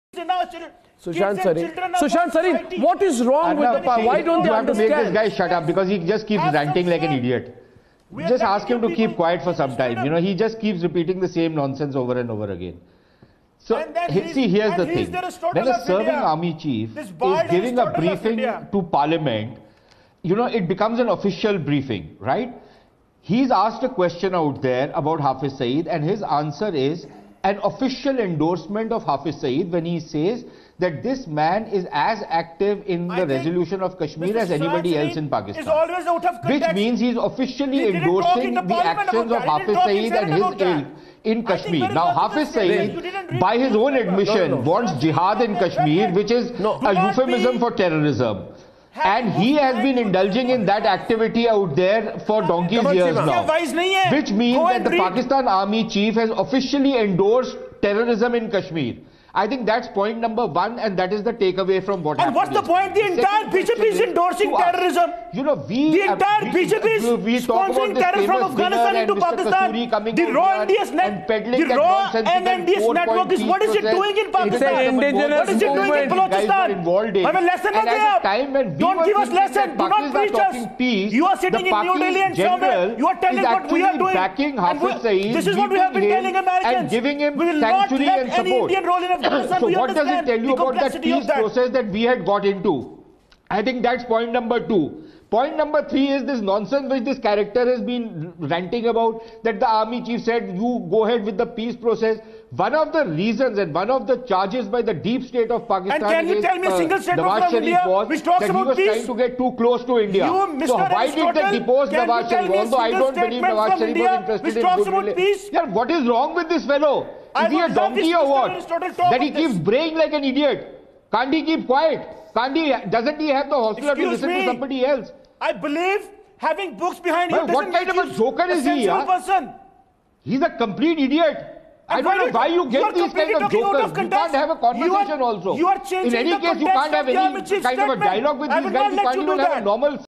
Arnab Goswami ripped Pakistan Apart
The takeaway from this is that the Pakistan Army has endorsed terrorism in Kashmir: Sushant Sareen, R. Contributor